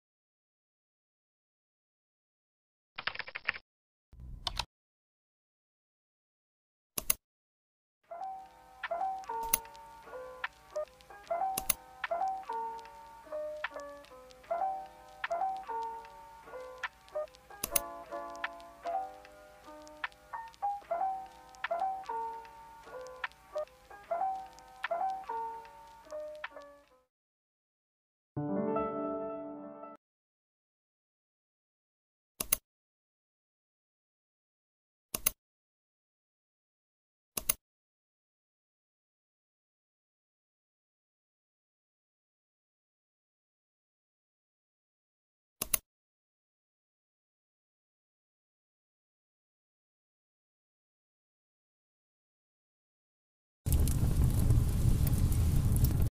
Aesthetic Intro & Outro Template (no text) || no credits needed
Hi! I am Vie. This is free to use but if you would like to credit me, kindly put this in your description box: desktop template by cozy vie

0:00-0:26 - w/ music
0:27-0:54 - no music

1. utilize screen recorder
2. copy and paste the link to any YT video downloader

» music

The images I used (folders, music player, notepad, et cetera) are ones I created using ibis Paint x and Canva. I edited the video in CapCut. I do not steal/grab other's work of art.